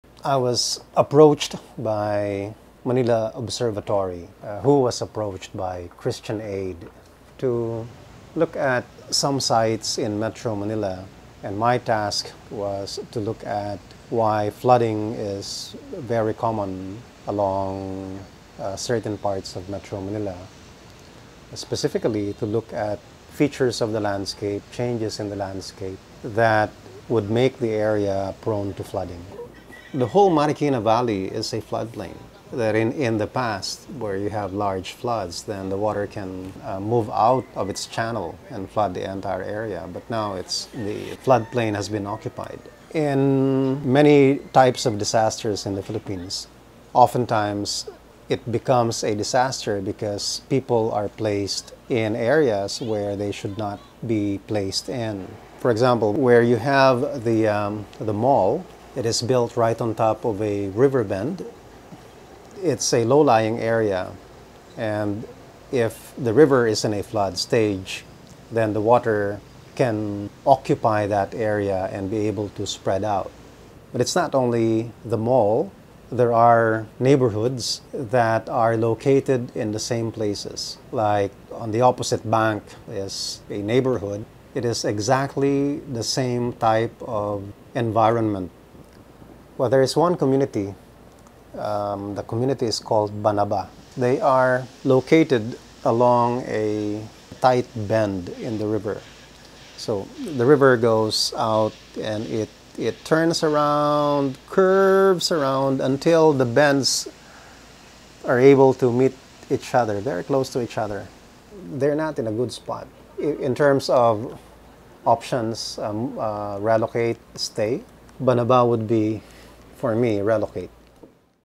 Big River Rising: Part 2 - 'Choke Points'
Professor Fernando Siringham from UP marine Science Institute talks about hazards and constrictions along the Marikina River that can lead to flooding in Manila.